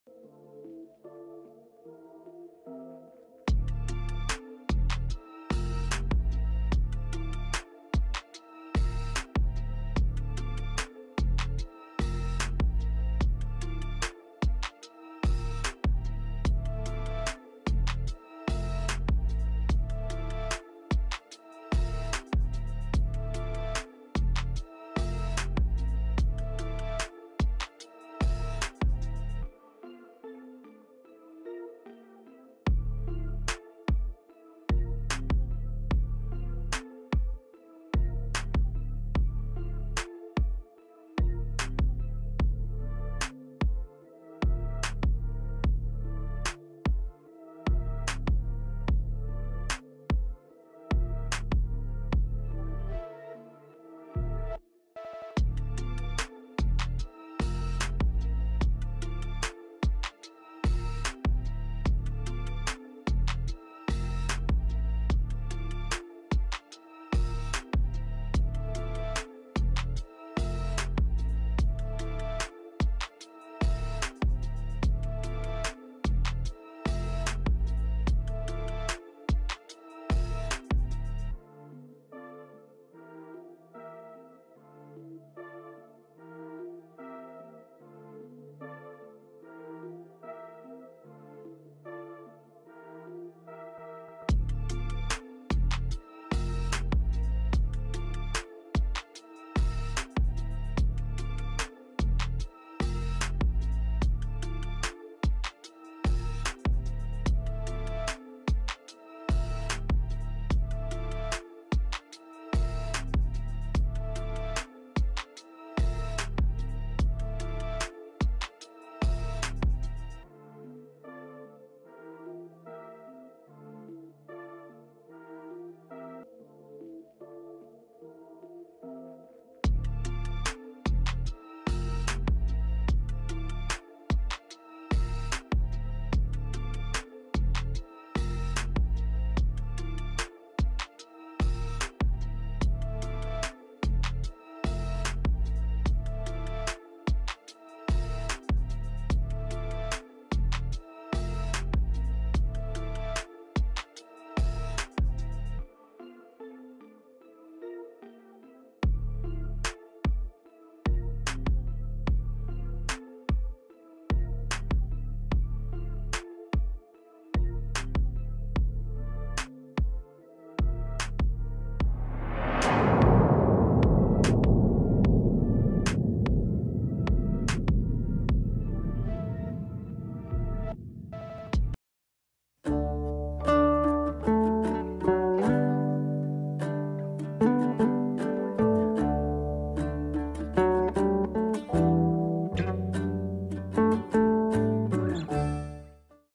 The Power of Intermediate Check | Bressy vs Arjun | Titled Tuesday Blitz 2023
Theo Bressy vs Arjun Erigaisi
Round-1.8
Titled Tuesday Blitz December 5 Late 2023